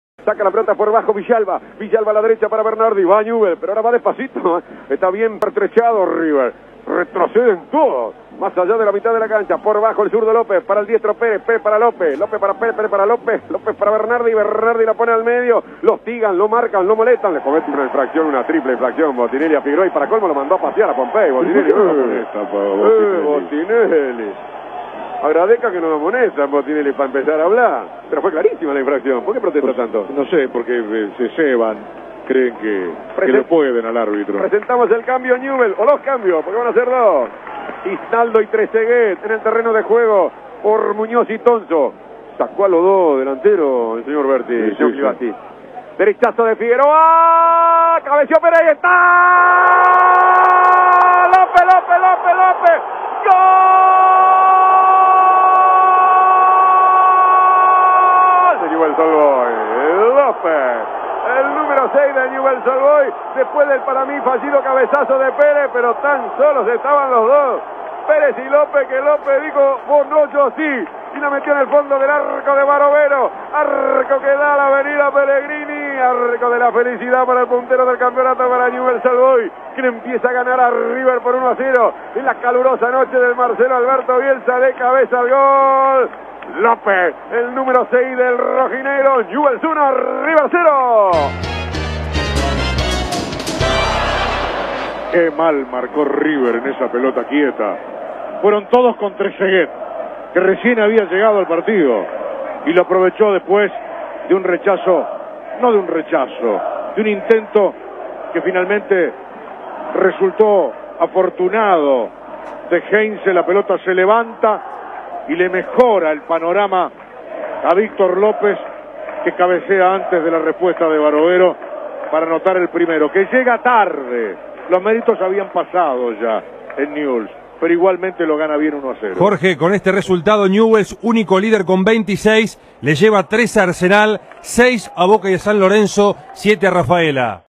Newell's 1 River 0 (Relato Jorge Arcapalo) Torneo Inicial 2013 (13/10/2013)
El gol del partido con relato de Jorge Arcapalo Am 590 Radio Continental